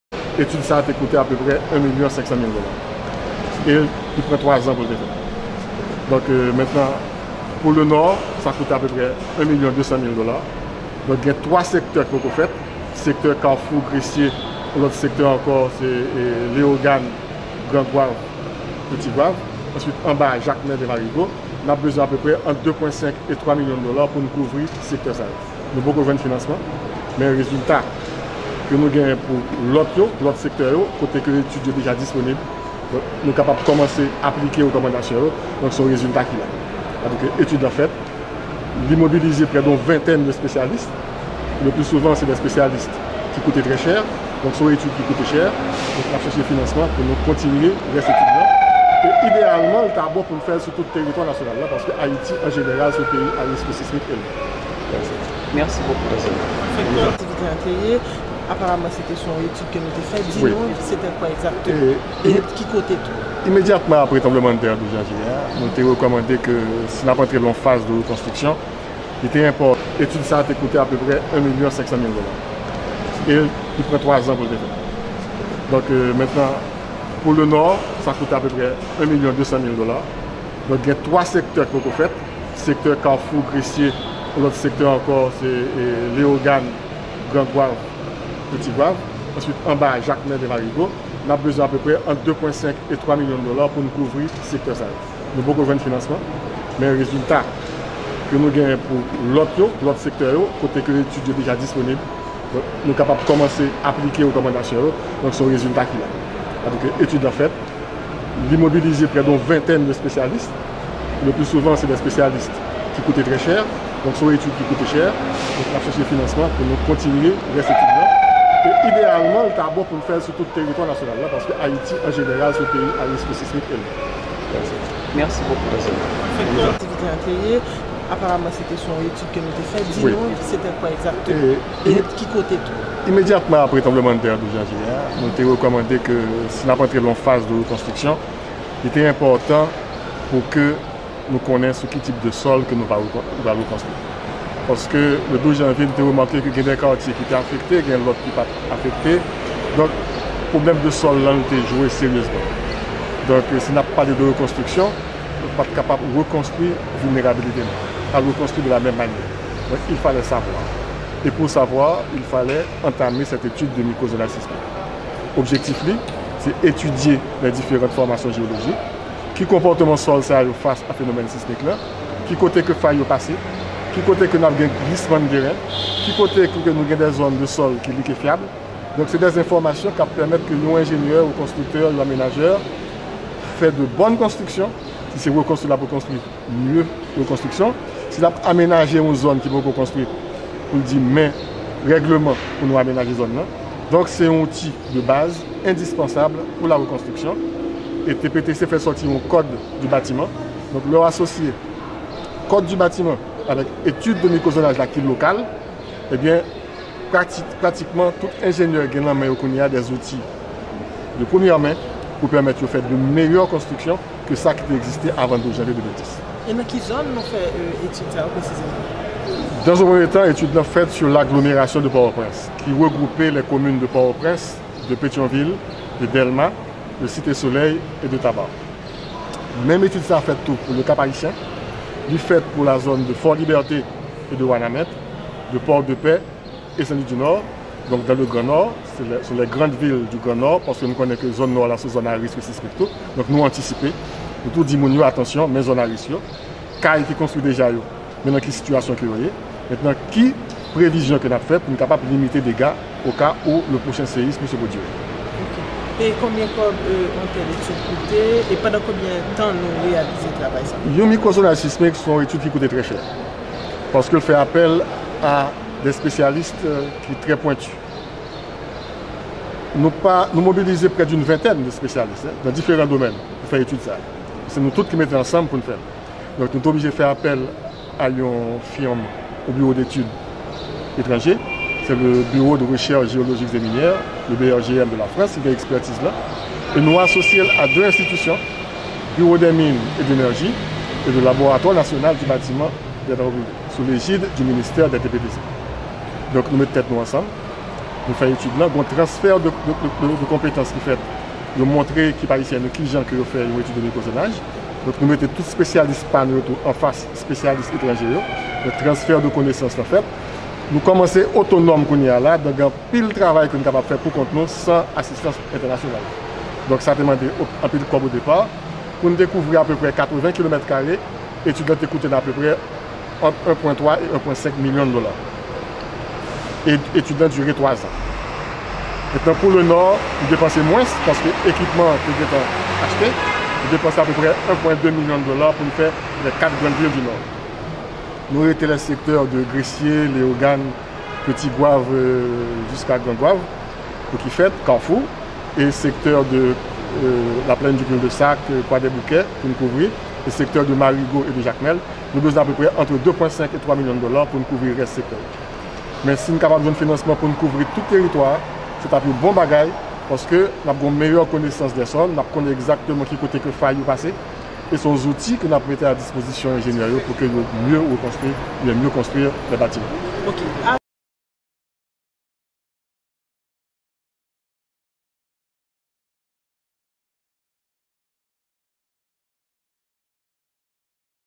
L'ingéneiur Géologue Claude PREPTIT présente le « microzonage sismique de l’agglomération de Port au
Connaître la nature de nos sols pour réduire la vulnérabilité des bâtiments
Port-au-Prince (24 Janvier 2017, Le Nouvelliste) C’est au centre de convention de la Banque de la République d'Haïti, à l’architecture contemporaine, au bas de la ville dans le centre historique de la capitale, que le ministère des Travaux publics a présenté l’étude sur le « microzonage sismique de l’agglomération de Port-au-Prince », mardi 24 janvier. Une étude qui révèle des connaissances, jusque-là ignorées, sur les formations des sols et leurs mouvements au moment d’un séisme ainsi que les mesures à prendre pour réduire les vulnérabilités des bâtiments.